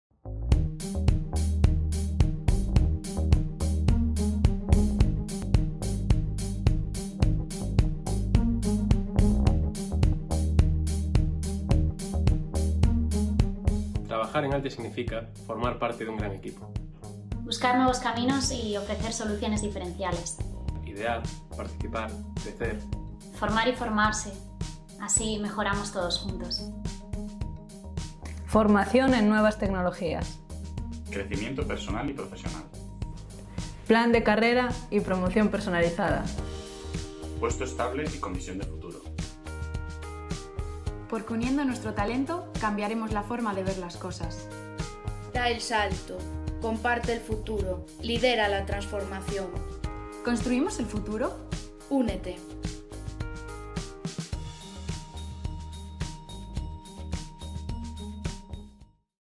Altia - Talentia Summit´20
ES | Empresa participante en la feria virtual de empleo y talento Talentia Summit´20. Con el objetivo de superar la incertidumbre, esta edición especial Covid-19 presenta ofertas de empleo en Galicia y en toda España para todos los participantes.
GL | Empresa participante na feira virtual de emprego e talento Talentia Summit´20. Co obxectivo de superar a incerteza, esta edición especial Covid-19 presenta ofertas de emprego en Galicia e en toda España para todos os participantes.